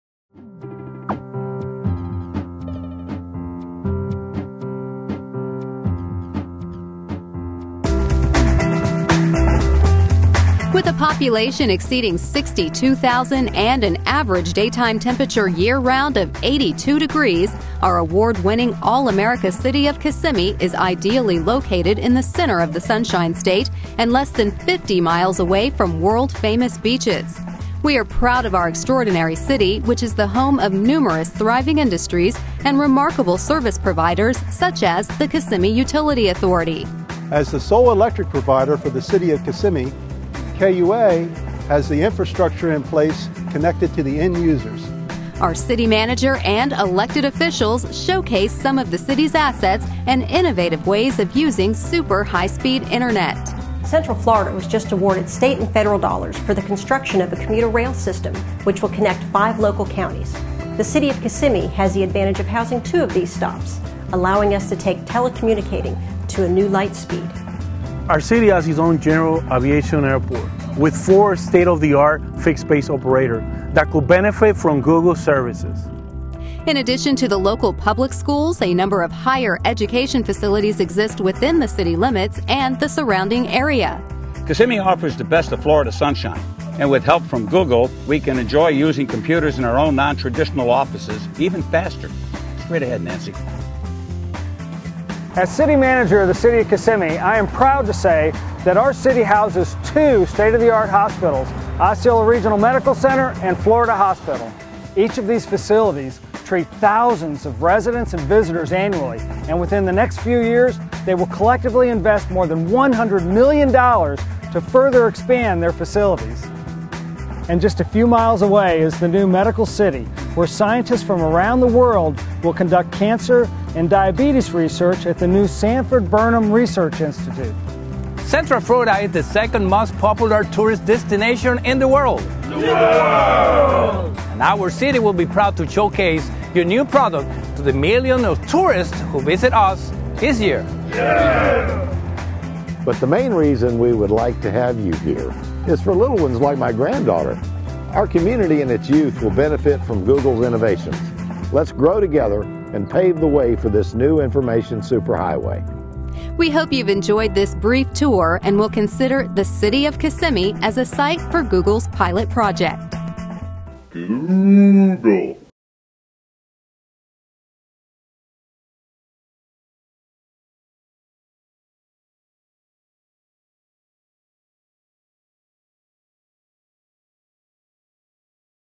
Kissimmee Gears up for Google
Google is accepting applications from cities across the country with the intent to install fiber optic broadband networks that will deliver speeds of one gigabit per second 100 times faster than existing Internet service